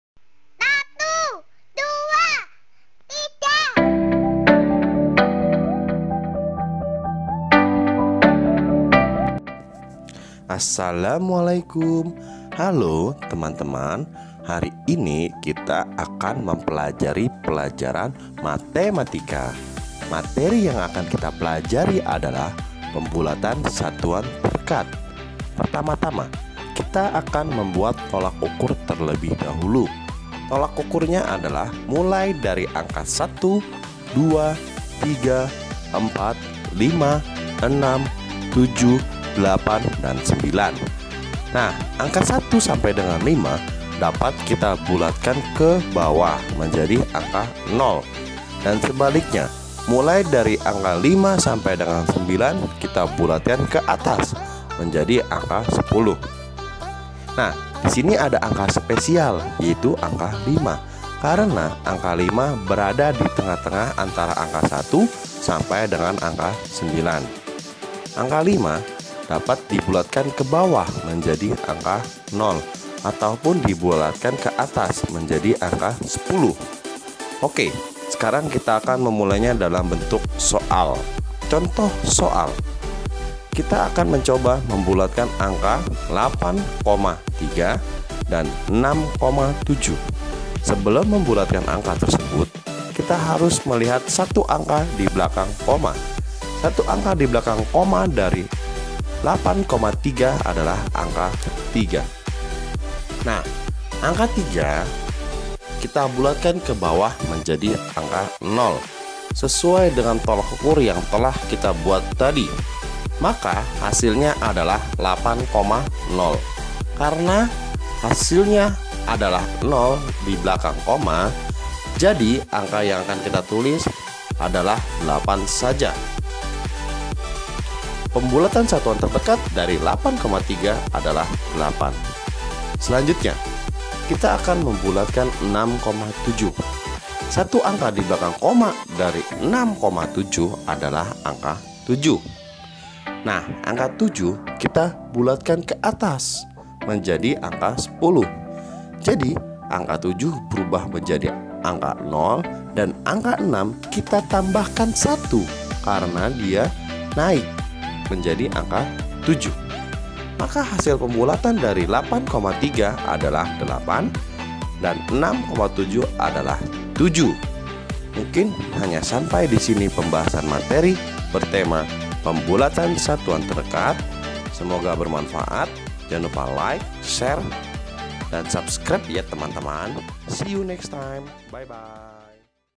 Ayo Belajar Pembulatan Satuan Terdekat Bersama
Video ini menjelaskan bagaimana kita mempelajari bagaimana cara kita dalam melakukan Pembulatan Satuan Terdekat, semoga video ini bermanfaat.

Selamat menonton